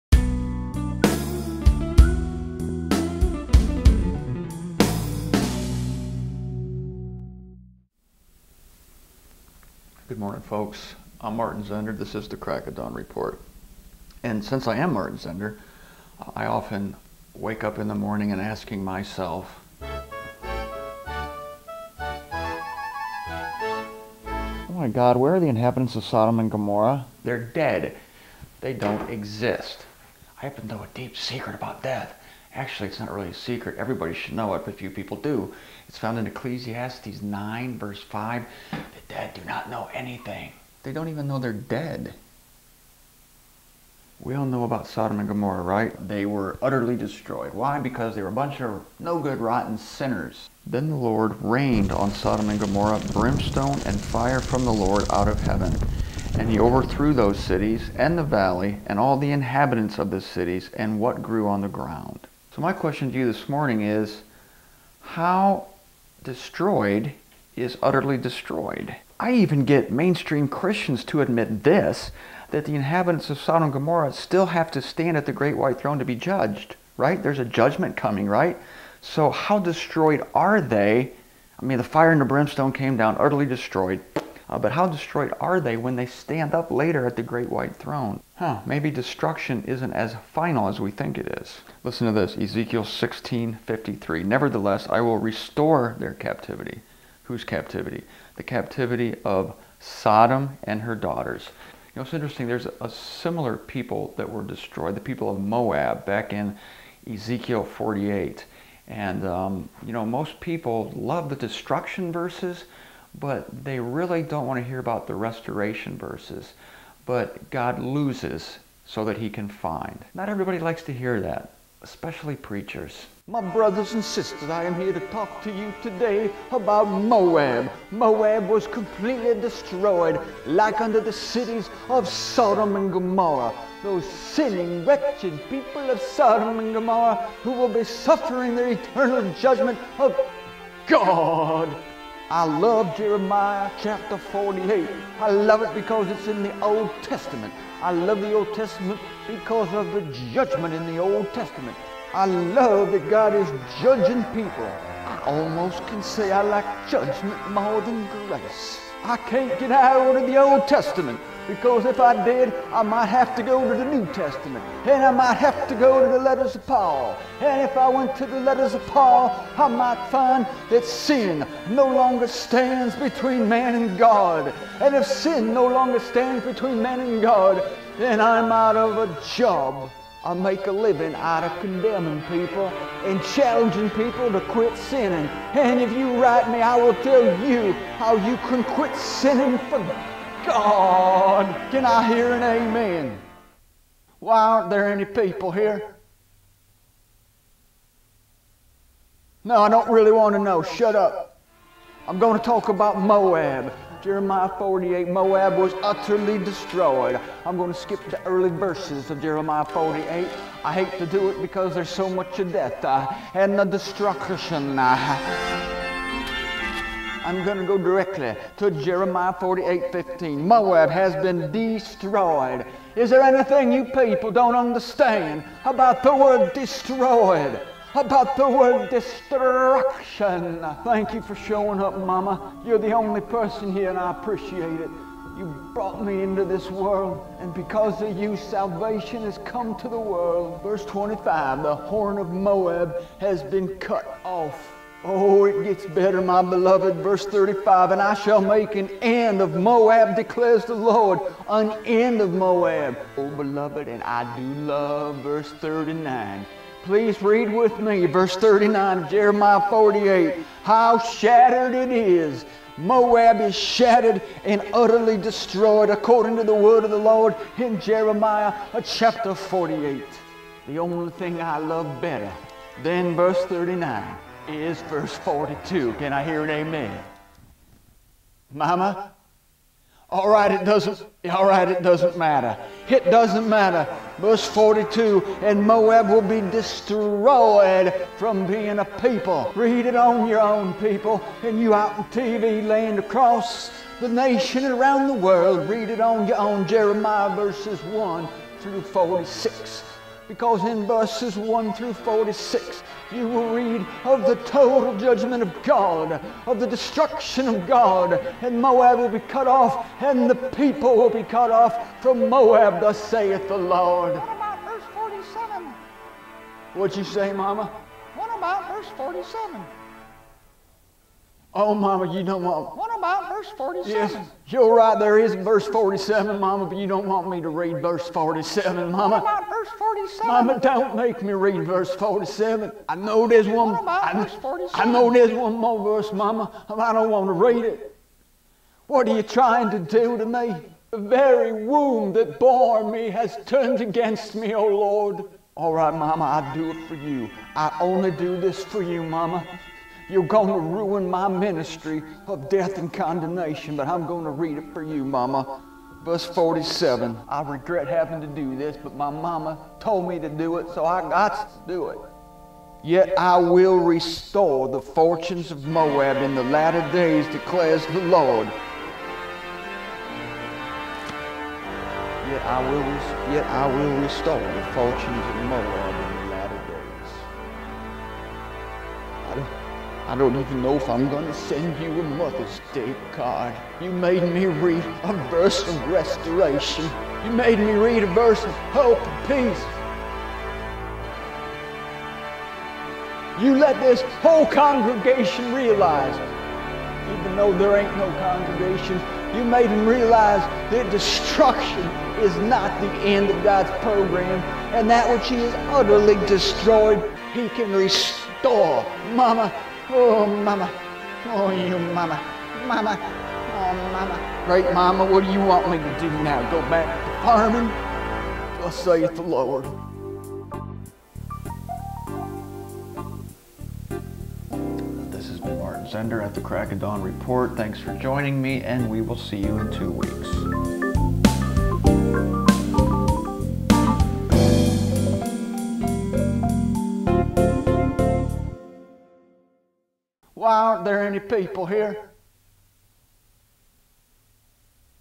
How Destroyed Was Sodom & Gomorrah?
We all know that God rained down fire and brimstone from heaven and utterly destroyed the cities of Sodom and Gomorrah and their inhabitants. And yet the inhabitants must yet rise to stand before God at the great white throne (Rev. 20:11-12). QUESTION: If these people rise again--even for judgment--how destroyed were they? Join Martin Zender and special guest, the Reverend Milt Hickle, as they look into the fate of a people who never heard of Jesus Christ.